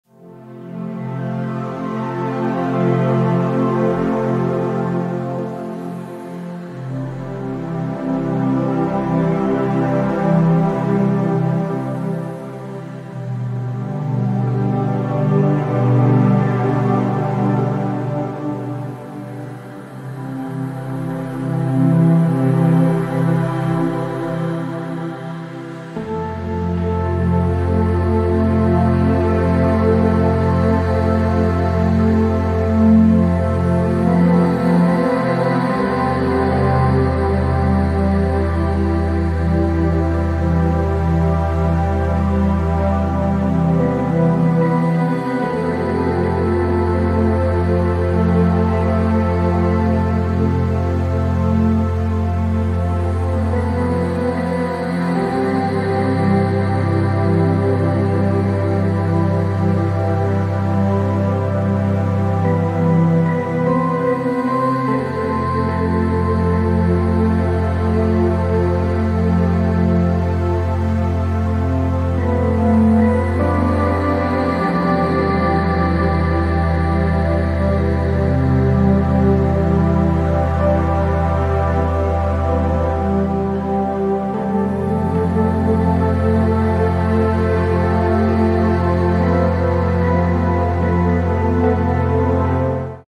CURSO DE FORMACION LECTORES REGISTROS AKASHICOS - EDDA SUAREZ
Ven a descubrir tu conexión interior , a accesar a tus propios registros akáshicos y también a ayudar a otros a descubrir su propio camino. Un Curso repleto de comprensión, sanación y apertura de camino en tu vida y de la quienes te rodean.